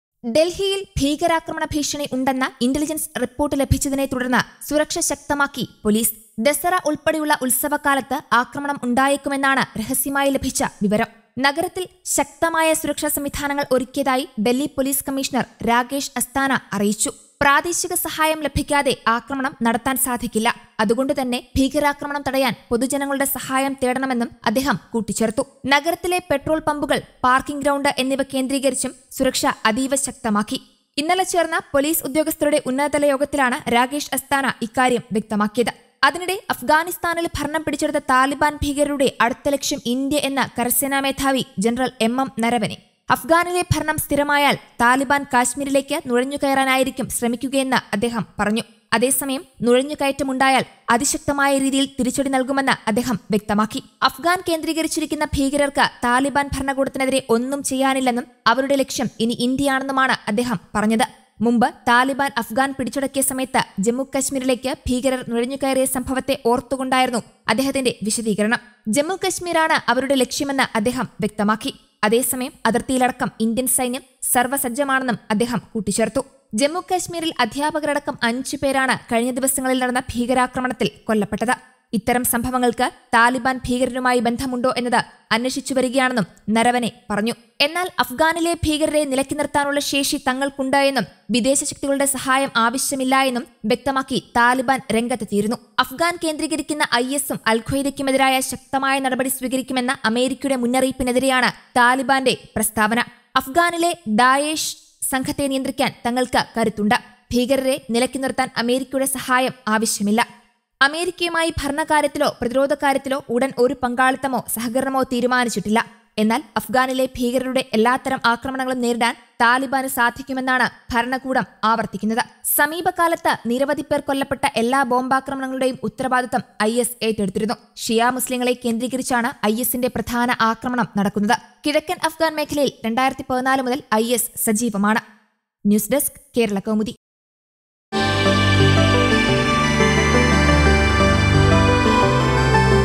Delhi Police on high alert after receiving inputs on terror attack during festive season
ഡല്‍ഹിയില്‍ ഭീകരാക്രമണ ഭീഷണി ഉണ്ടെന്ന ഇന്റലിജന്‍സ് റിപ്പോര്‍ട്ട് ലഭിച്ചതിനെ തുടര്‍ന്ന് സുരക്ഷ ശക്തമാക്കി പൊലീസ്.ദസറ ഉള്‍പ്പെടെയുള്ള ഉത്സവകാലത്ത് ആക്രമണം ഉണ്ടായേക്കും എന്നാതാണ് രഹസ്യമായി ലഭിച്ച വിവരം.
നഗരത്തില്‍ ശക്തമായ സുരക്ഷാ സംവിധാനങ്ങള്‍ ഒരുക്കിയതായി ഡല്‍ഹി പൊലീസ് കമ്മിഷണര്‍ രാകേഷ് അസ്താന അറിയിച്ചു. പ്രാദേശിക സഹായം ലഭിക്കാതെ ആക്രമണം നടത്താന്‍ സാധിക്കില്ല, അതുകൊണ്ടുതന്നെ ഭീകരാക്രമണം തടയാന്‍ പൊതുജനങ്ങളുടെ സഹായം തേടണമെന്നും അദ്ദേഹം കൂട്ടിച്ചേര്‍ത്തു.നഗരത്തിലെ പെട്രോള്‍ പമ്ബുകള്‍, പാര്‍ക്കിംഗ് ഗ്രൗണ്ട് എന്നിവ കേന്ദ്രീകരിച്ചും സുരക്ഷ അതീവ ശക്തമാക്കി. ഇന്നലെ ചേര്‍ന്ന പോലീസ് ഉദ്യോഗസ്ഥരുടെ ഉന്നതതല യോഗത്തിലാണ് രാകേഷ് അസ്താന ഇക്കാര്യം വ്യക്തമാക്കിയത്. അതിനിടെ അഫ്ഗാനിസ്ഥാനില്‍ ഭരണം പിടിച്ചെടുത്ത താലിബാന്‍ ഭീകരരുടെ അടുത്ത ലക്ഷ്യം ഇന്ത്യ എന്ന് കരസേനാ മേധാവി ജനറല്‍ എം.എം. നരവനെ. അഫ്ഗാനിലെ ഭരണം സ്ഥിരമായാല്‍ താലിബാന്‍ കാശ്മീരിലേക്ക് നുഴഞ്ഞു കയറാനായിരിക്കും ശ്രമിക്കുകയെന്ന് അദ്ദേഹം പറഞ്ഞു